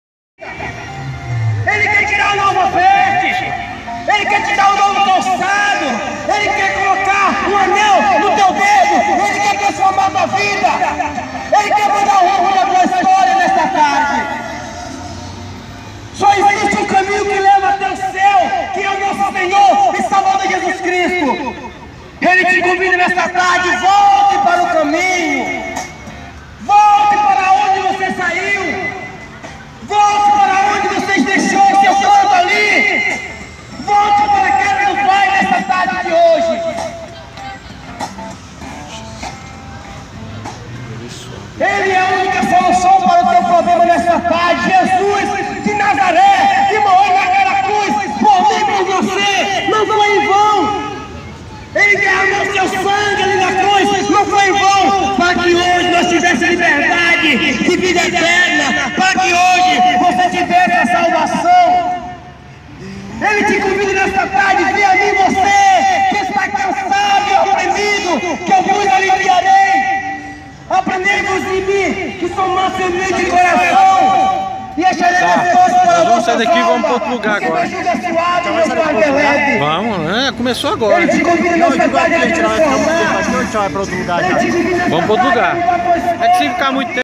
missão resgate eraiton ministrando a palavra de Deus
evangelismos missão resgate eraiton ministrando a palavra de Deus na praça da moça Diadema